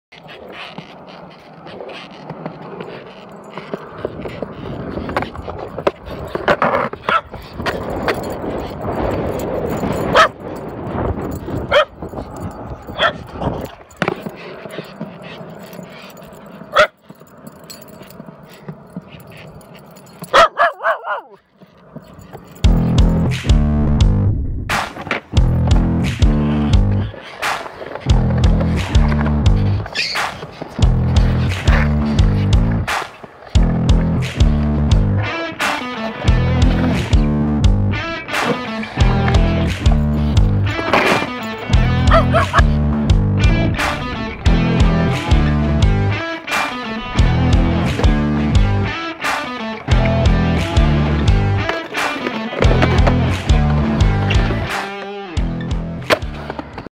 Mrs. Speedlab, Natty and I enjoyed the 70 degree Feb. weather yesterday at HQ, filmed with the sm1
Mrs. Speedlab, Natty and I enjoyed the 70 degree Feb. weather yesterday at HQ, filmed with the sm1lens using the sm1handle